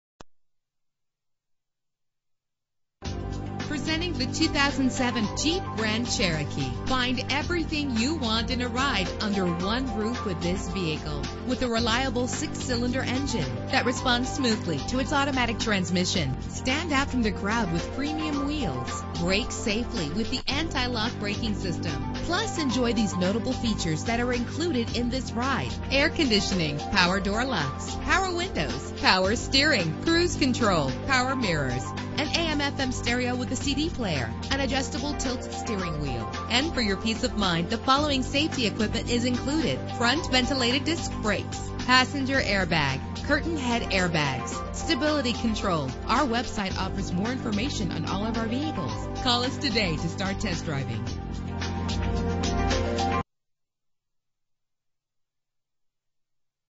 2007 Jeep Grand Cherokee - Braintree MA
Year: 2007
Make: Jeep
Model: Grand Cherokee
Trim: Laredo
Engine: 3.7 liter 6 cylinder 12 valve
Transmission: Automatic
Color: Red
Mileage: 33448
Address:
75 Granite Street Route 37